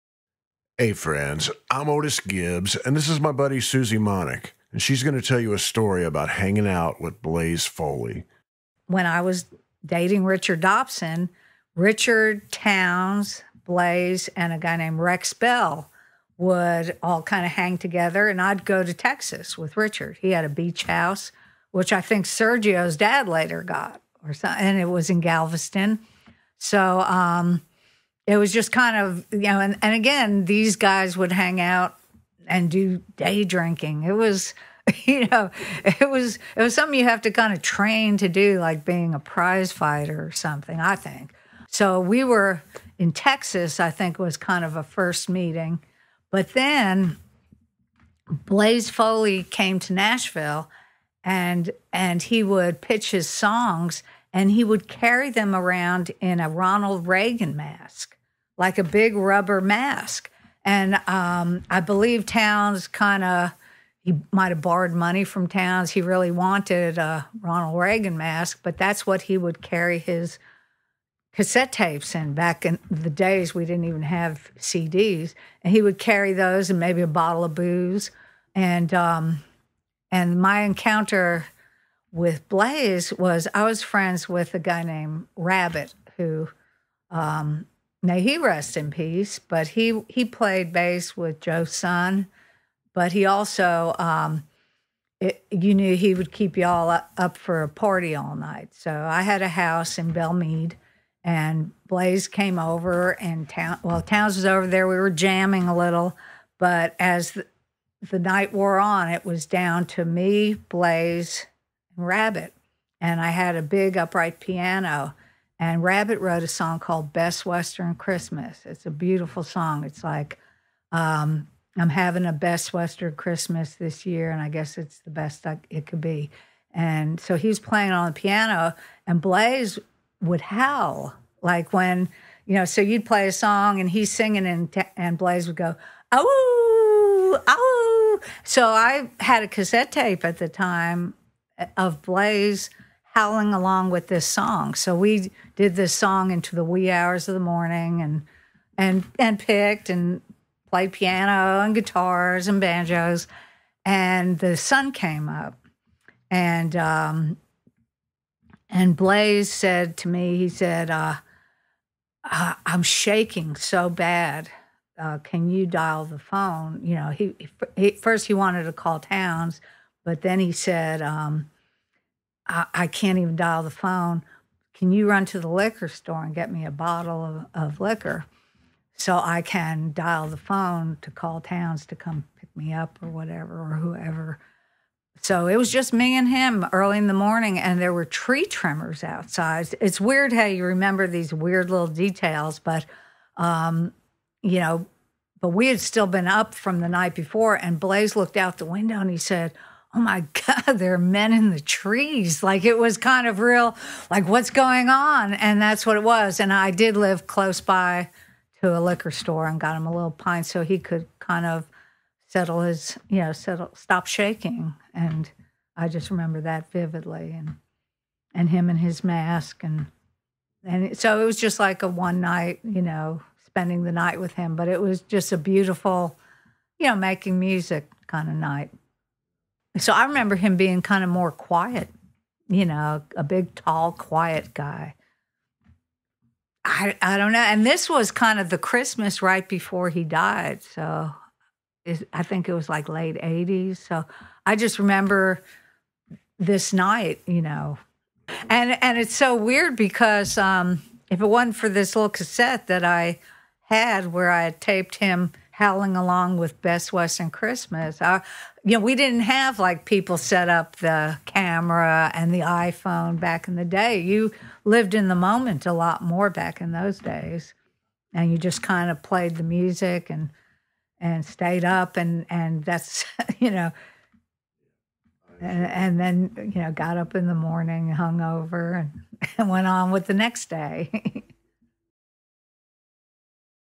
Blaze Foley Had A Mask Full Of Demos
Susie Monick shares a story about hanging out and playing music with Blaze Foley,

Thank you for helping make this channel possible.

Blaze Foley
Susie Monick